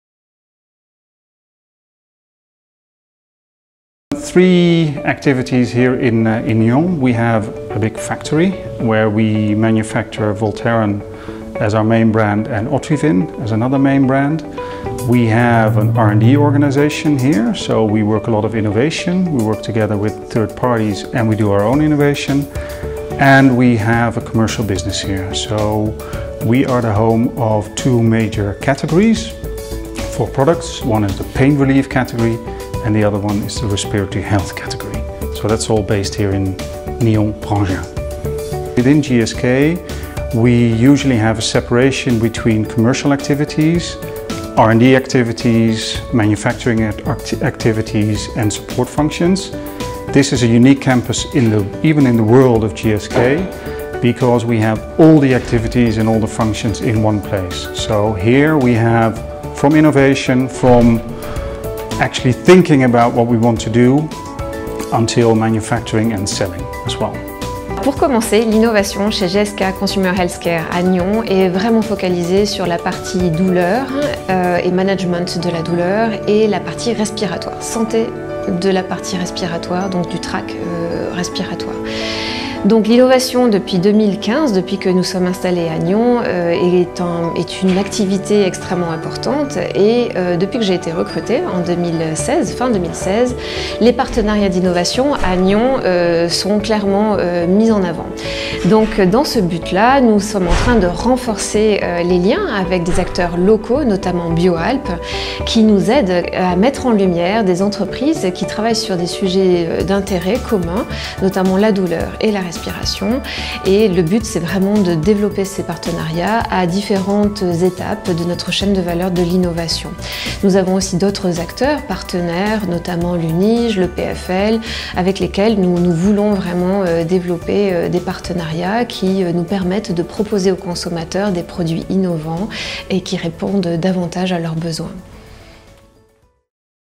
4à6 BioAlps at GlaxoSmithKline Consumer Healthcare (VD)
In partnership with the Greater Geneva Bern area team, we are happy to present to you the video of our 4à6 event that took place in June 2019 at GSK Consumer Healthcare in Nyon, Vaud.

BioAlps’ 4à6 are a series of events organized in each of the 7 cantons of western Switzerland and are a perfect opportunity to visit some of the major regional actors in the biotech, medtech, pharma and digital health fields.